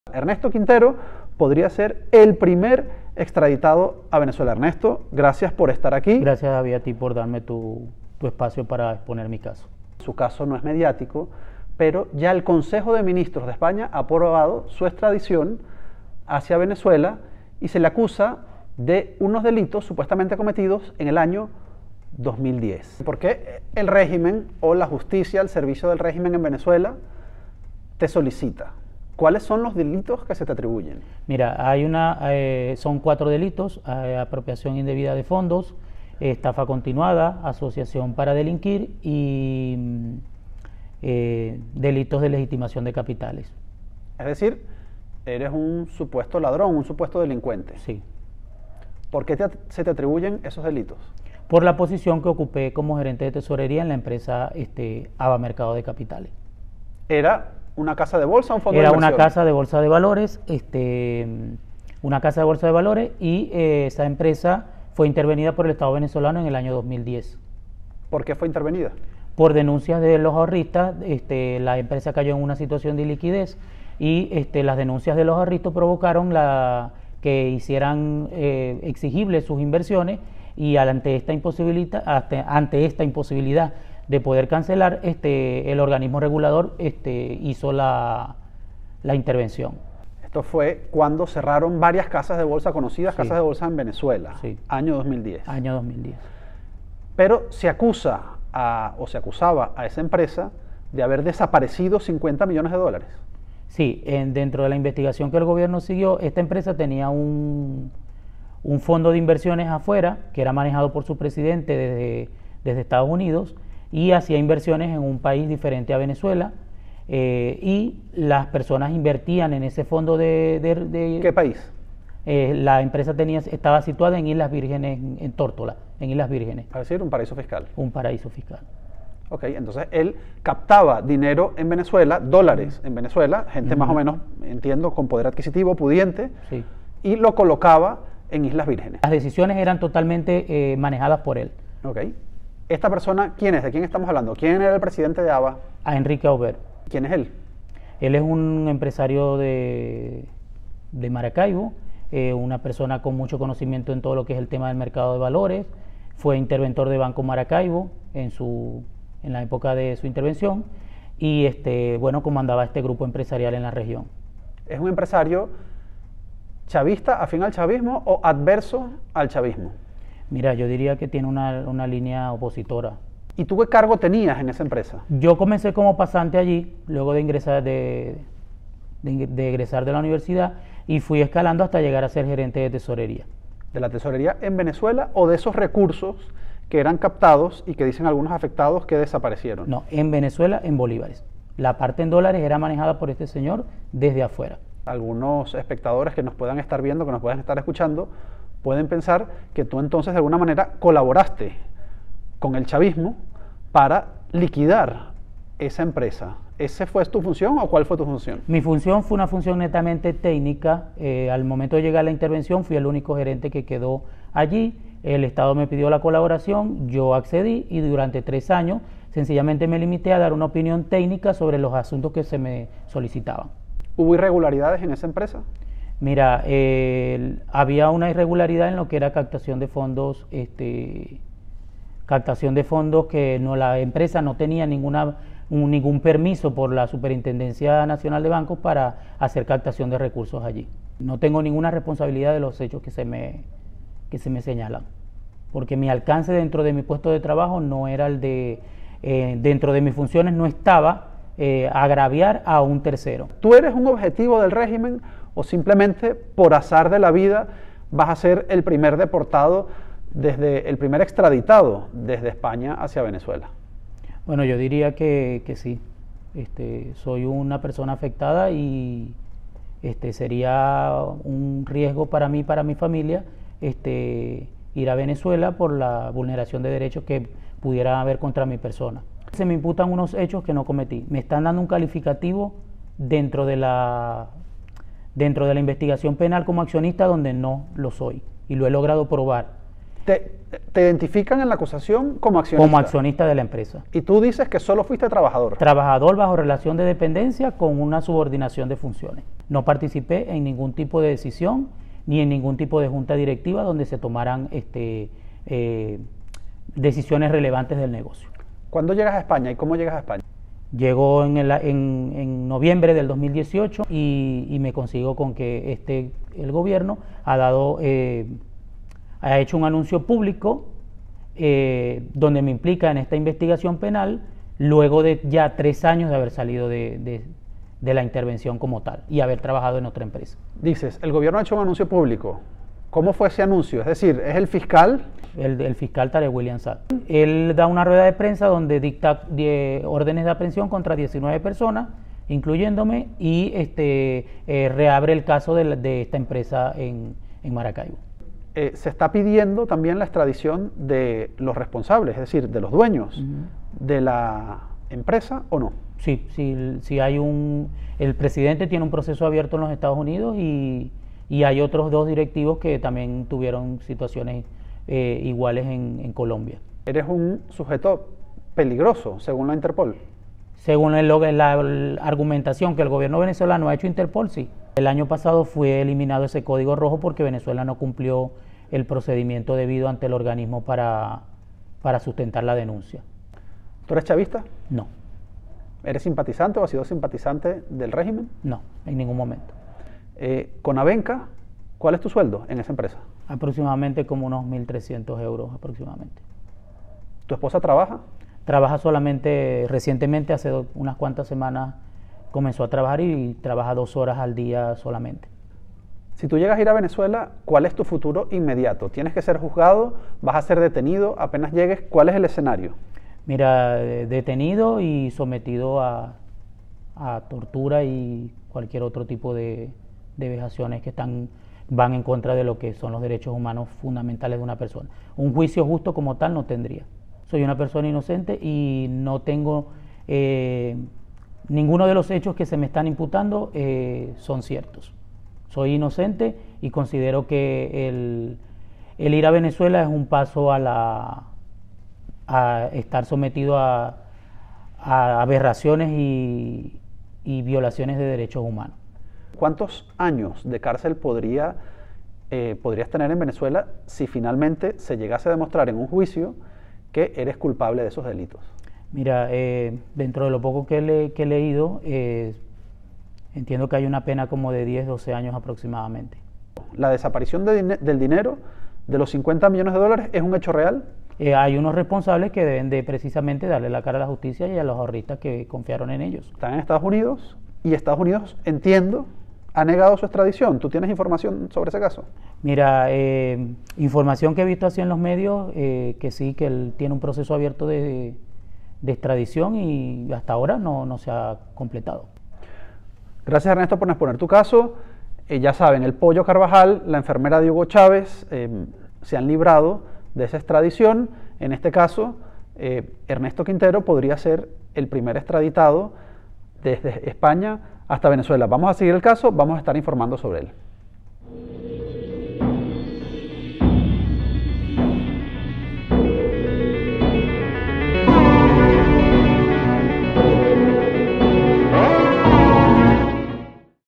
"Me van a torturar": Habla el hombre que España entregará al chavismo
Ernesto Quintero, un trabajador de una empresa de envíos de mercancías a Venezuela, será entregado por España a Venezuela.
El chavismo lo acusa de participar en un desfalco de 50 millones de dólares en la casa de cambio ABA Mercado de Capitales. Quintero no ha tenido dinero ni para pagar abogados privados. Ha tenido que recurrir al abogado de oficio. Su esposa trabaja como limpiadora en unas oficinas bancarias apenas dos horas al día.
¿Por qué España entrega a un hombre a la dictadura venezolana?